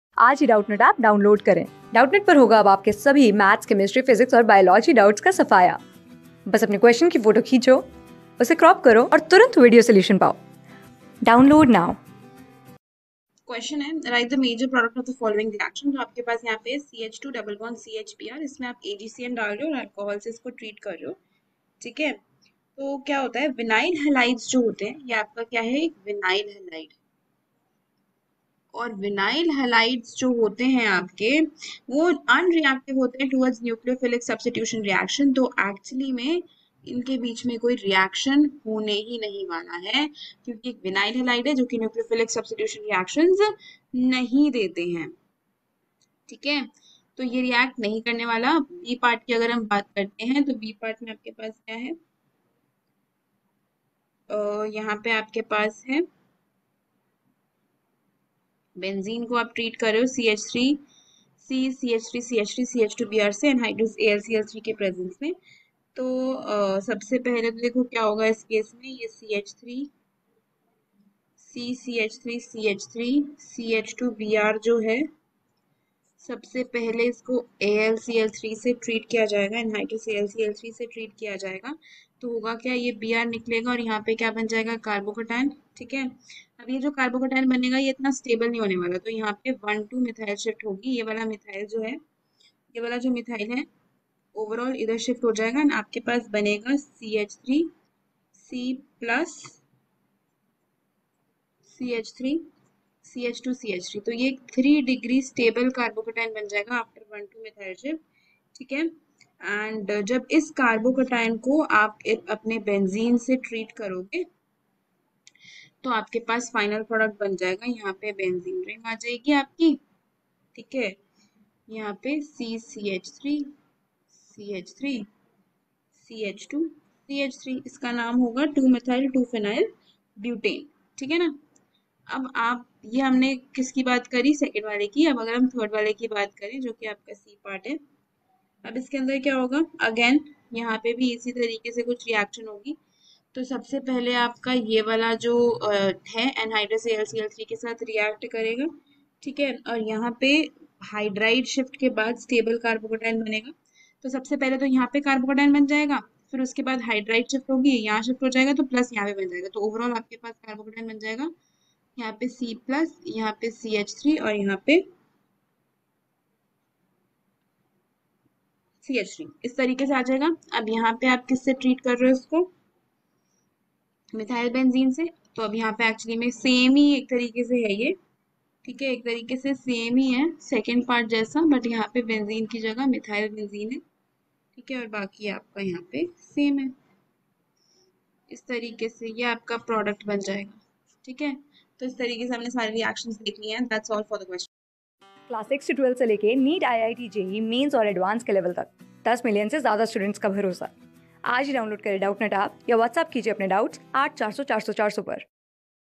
Write the major product of the following reaction: (a) CH_(2)=CHBr underset(alcohol)overset(AgCN...
Write the major product of the following reaction: (a) CH_(2)=CHBr underset(alcohol)overset(AgCN)to.
Class: 12
Subject: CHEMISTRY
Chapter: HALOALKANES AND HALOARENES
Board:CBSE

👉 Have Any Query? Ask Us.